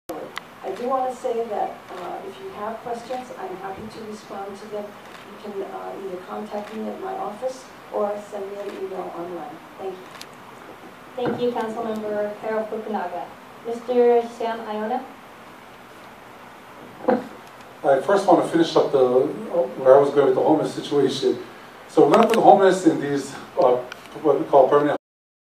CAROL FUKUNAGA
Rematch for Carol, Sam, and Steve, but for Joli Tokusato, a new challenge.